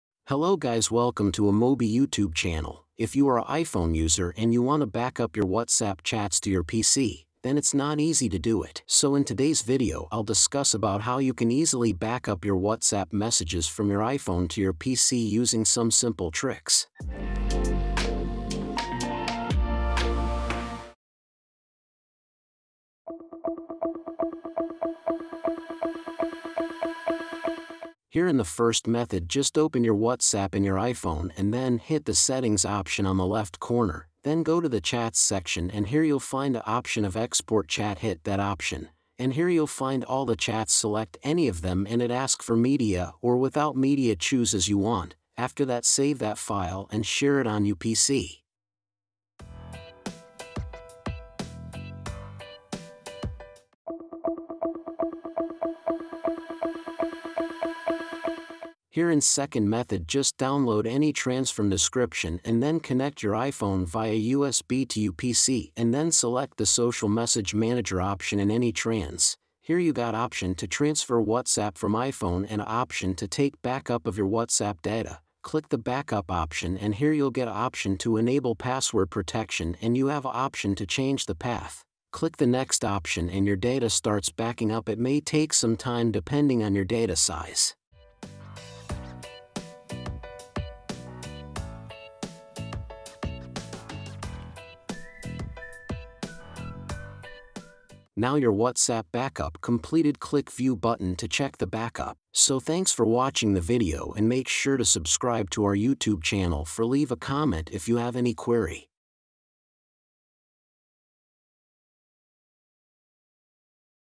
[2 Ways] How to Backup WhatsApp from iPhone to PC 2025
WhatsApp maintains a lot of your memories. But do you know how to keep them forever? In other words, would you like to know how to back up WhatsApp from iPhone to PC?

Here, we’d like to share with you several efficient ways how to back up WhatsApp from iPhone to PC. Let’s figure it out!
00:00 Intro
00:24 Backup WhatsApp from iPhone to PC with Email
00:54 Backup WhatsApp from iPhone to PC without iCloud